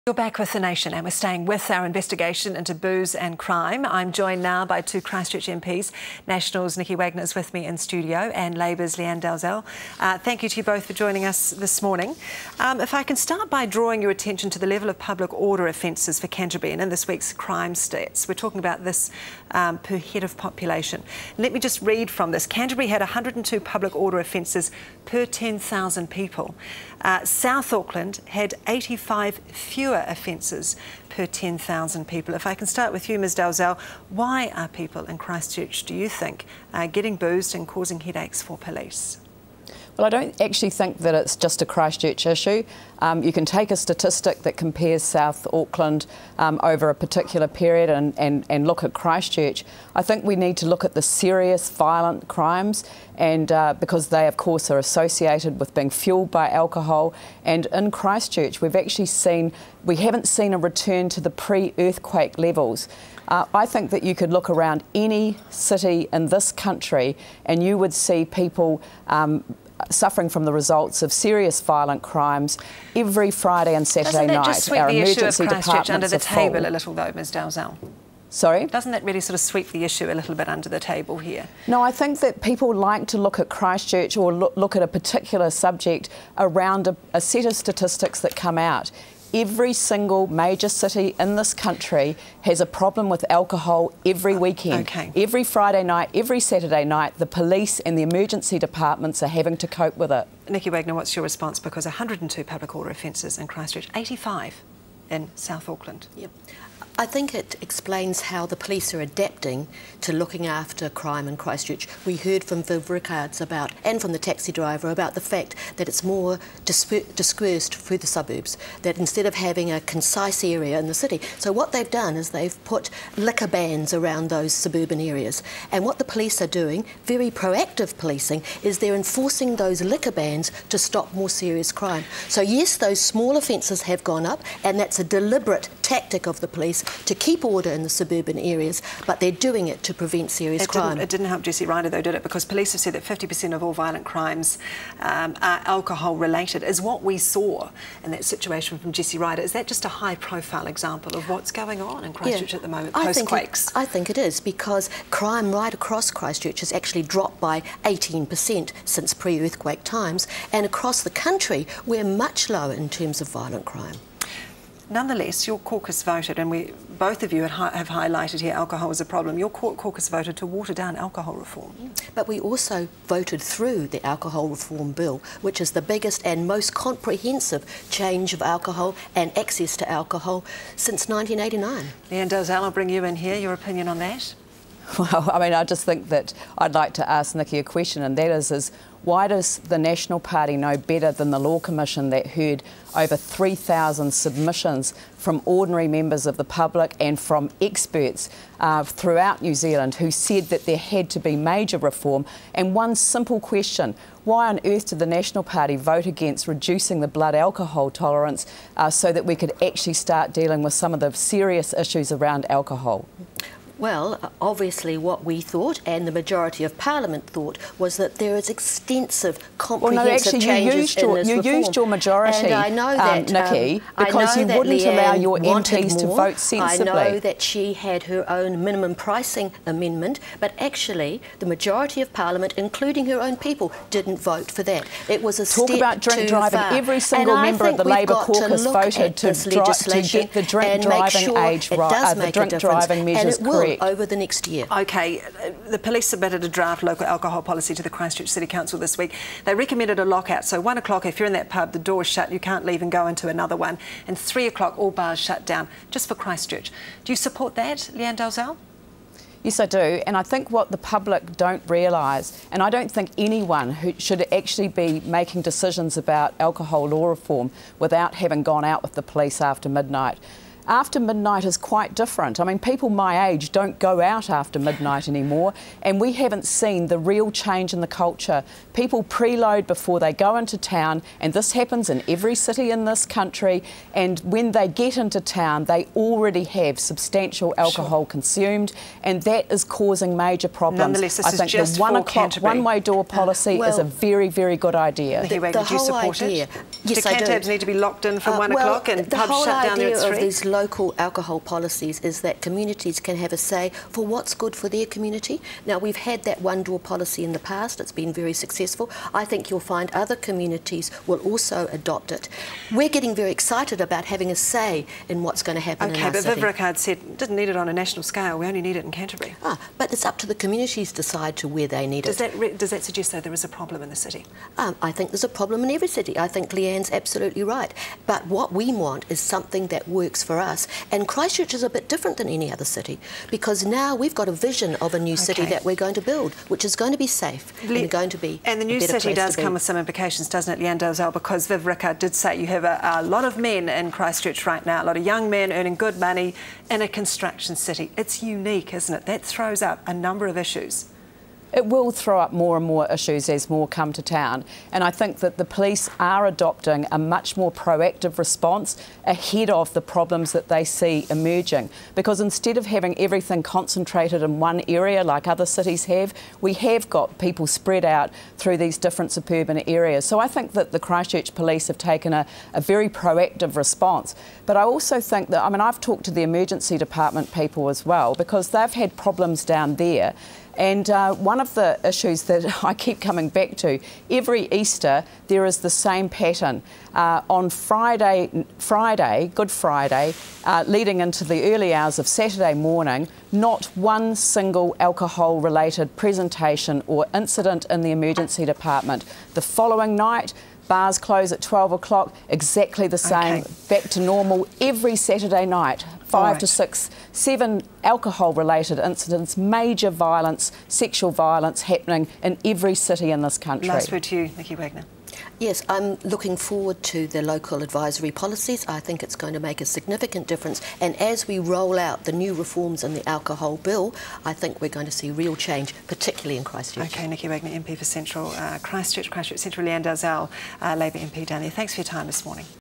MPs defend Christchurch's growing crime statistics
The 2012 Police crime statistics have revealed a rise in Christchurch's crime rate, particularly on the issue of public order offences, however two Christchurch MPs say the city has no more alcohol related crimes than the rest of New Zealand.

Lianne Dalziel, Labour MP Christchurch East says that the issues faced by police in the city are the same as those witnessed across New Zealand.

"I think that people like to look at Christchurch or look at a particular subject around a set of statistics that come out.  Every single major city in this country has a problem with alcohol every weekend, every Friday night every Saturday night the Police and the Emergency Departments are having to cope with it," Ms Dalziel says.

National MP for Christchurch Central, Nicky Wagener says that the higher crime rate seen in Christchurch are a reflection of police managing a more dispersed drinking population.

"So what they've done is they've put liquor bans around those suburban areas, and what the Police are doing, fairly proactive policing is they're enforcing those liquor bans to stop more serious crime.  So yes those small offences have gone up, and that's a deliberate tactic of the Police to keep order in the suburban areas, but they're doing it to prevent serious crime," Ms Wagener says.